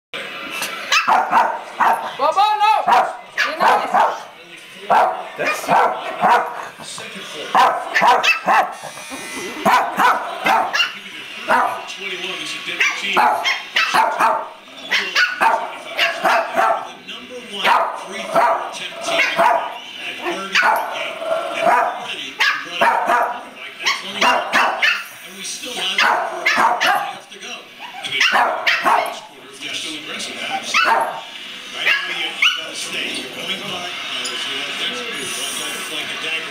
Rambo loves Mr. Bobo
Our new puppy Rambo barks for the first time, and decides to "play" with Mr. Bobo, much to his displeasure.